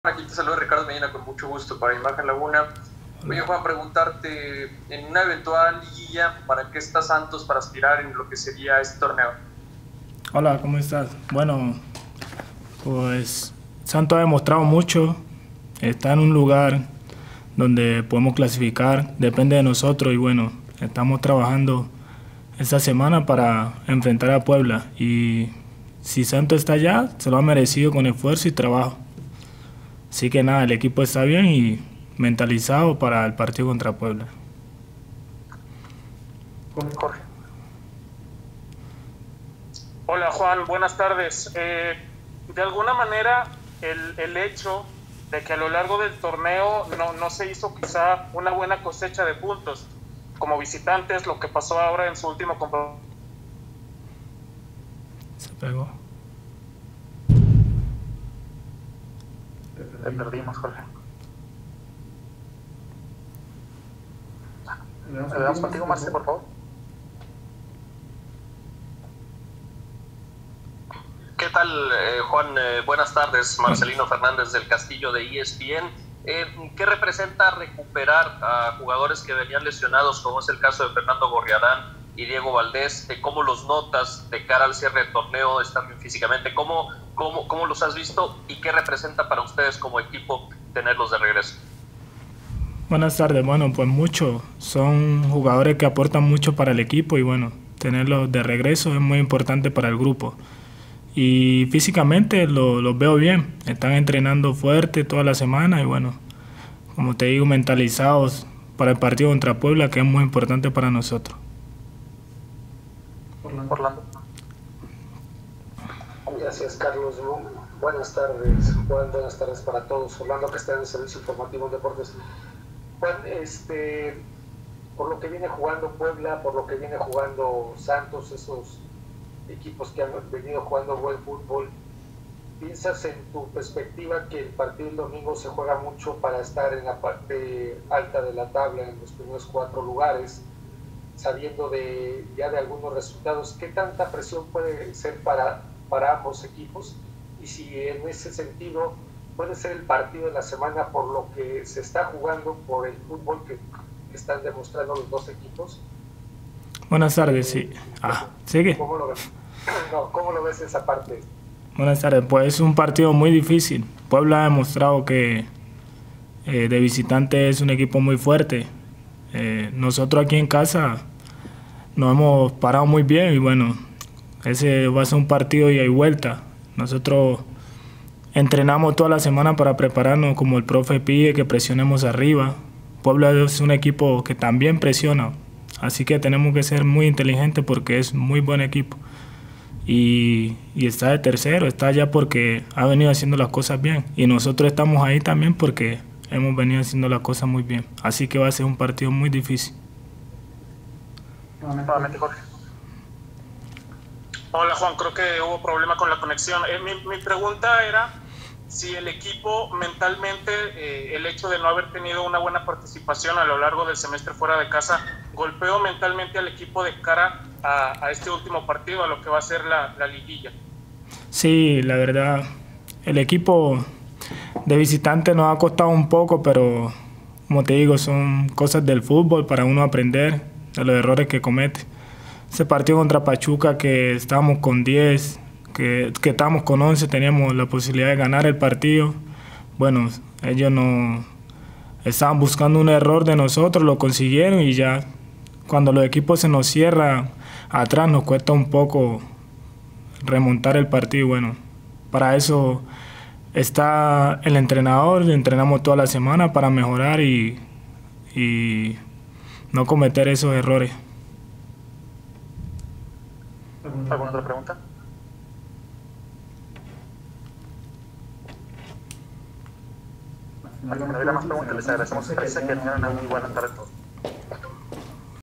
CONFERENCIA | El equipo esta bien y mentalizado para el partido contra Puebla. Juan Otero.
📹 | Nuestro Guerrero Juan Otero tuvo la oportunidad de atender a medios de comunicación y esto fue lo que nos dijo.